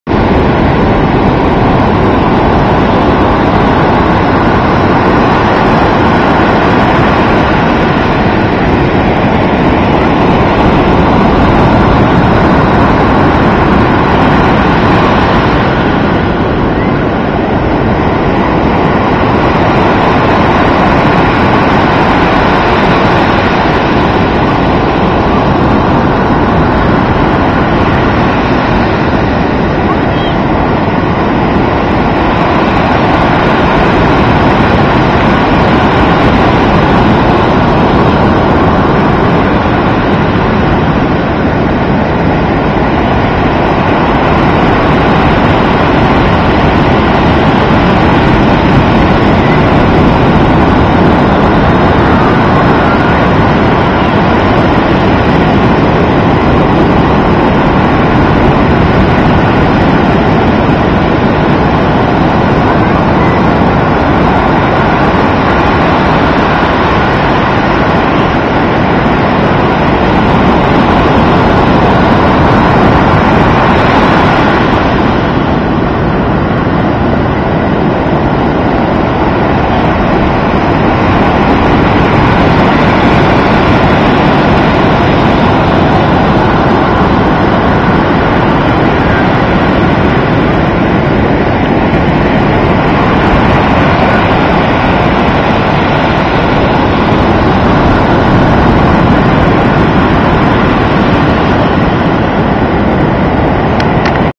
Just some random Ed Hartel Clip
Enjoy a 1:43 segment of the sunset from Crystal Pier, Pacific Beach, in San Diego - Recorded on September 10, 2011